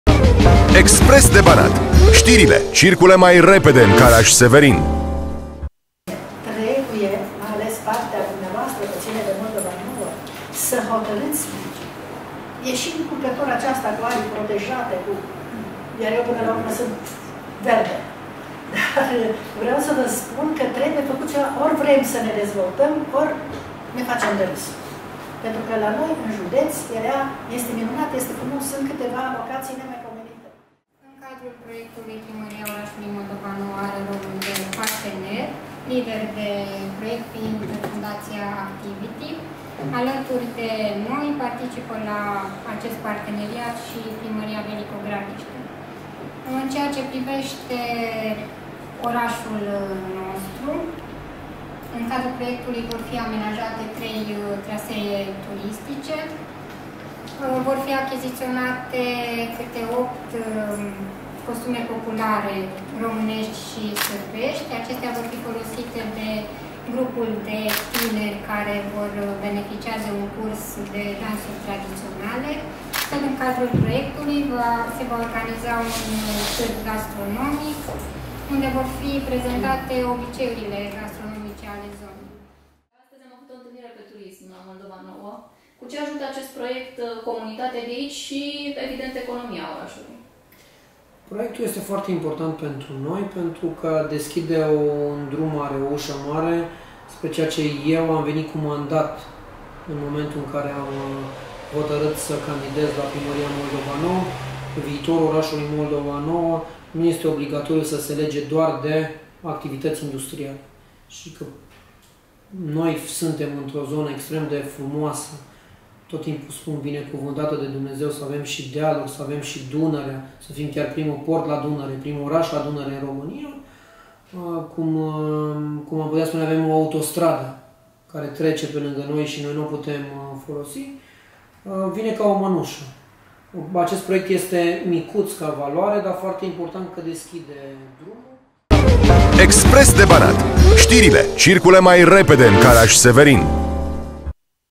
Proiect transfrontalier România-Serbia pentru promovarea turismului
Peisajele unice din Moldova Nouă și obiceiurile din zonă vor fi promovate prin intermediul unui proiect transfrontalier. Prima conferință de prezentare s-a derulat marți, 29 august, la sediul administrației publice locale.